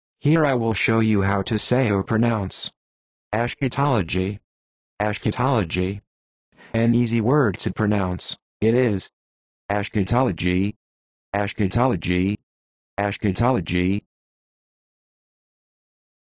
How to Say or Pronounce Eschatology
This video shows you how to say or pronounce Eschatology.

How accurate does it say Eschatology? How would you say Eschatology?